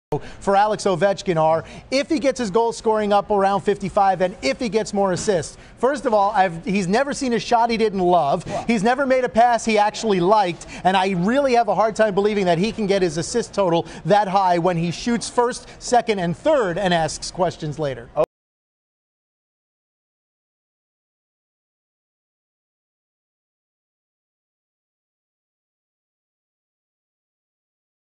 John Giannone Disses Alex Ovechkin Out (Really Bad)
Agreed ..
hes a good player
but that comment my friend's its true alright admit it caps fan